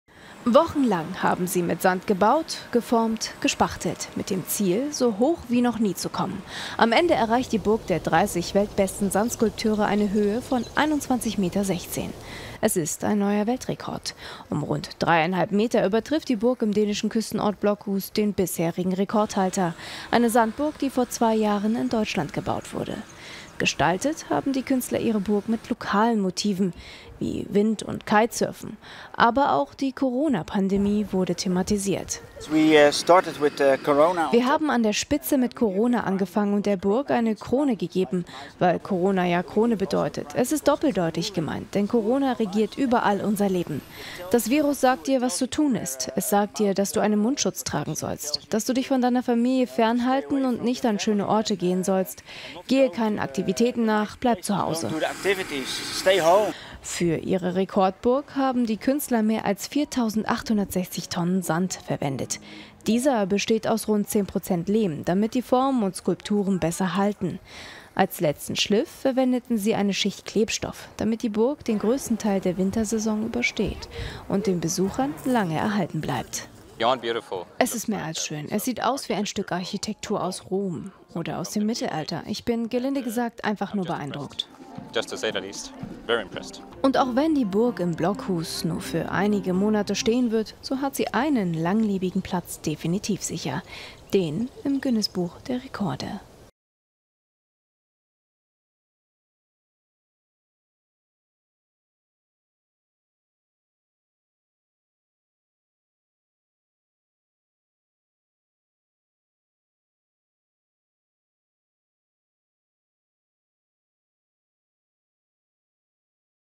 NEUER WELTREKORD: 4860 Tonnen dänischer Sand – höchste Sandburg der Welt gebaut
In dem kleinen Badeort Blokhus an der dänischen Nordseeküste gibt es einen neuen Weltrekord zu bestaunen. Mit einer Höhe von 21,16 Metern hat es dort eine Sandburg ins Guinness-Buch geschafft.